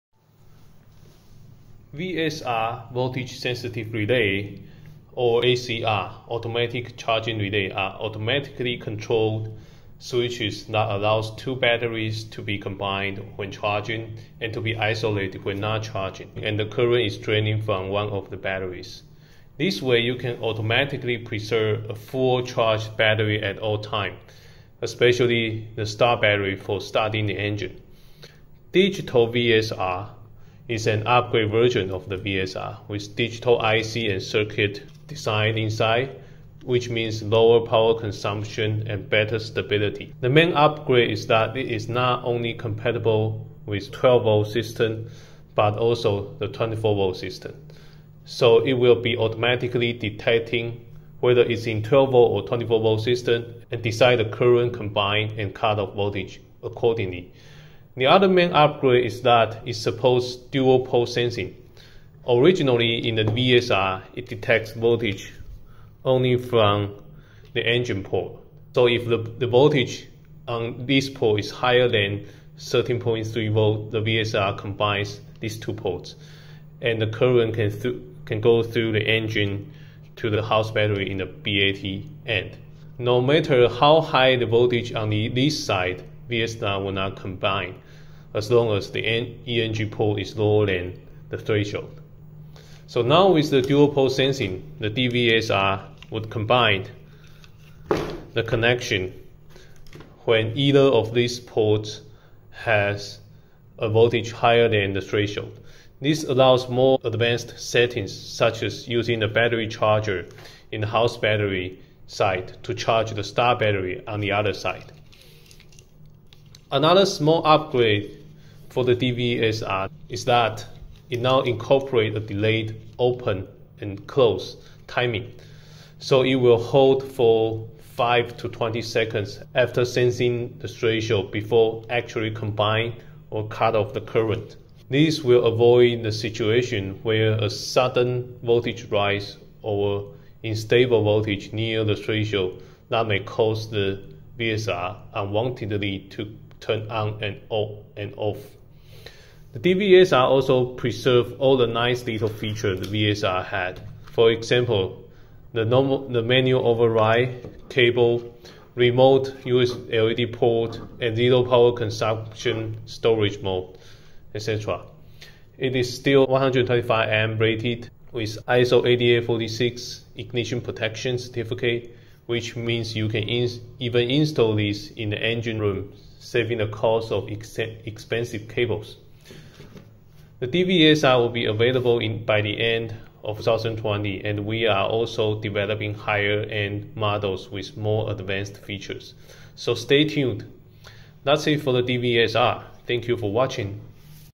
BF452 DVSR (Digital Voltage Sensitive Relay
VSR (Voltage Sensitive Relay) or ACR (Automatic Charging Relay) are automatically controlled switches that allow two batteries to be combined when charging, and to be isolated when not charging and current is draining from one of the batteries. When used with battery switches, VSR helps to reserve a fully charged battery at all time, avoid the dead battery and possibility of towing on the sea. D-VSR is an upgraded Digital version of VSR with enhanced features like 12/24V system support, dual-port sensing, much lower standby power consumption and better reliability.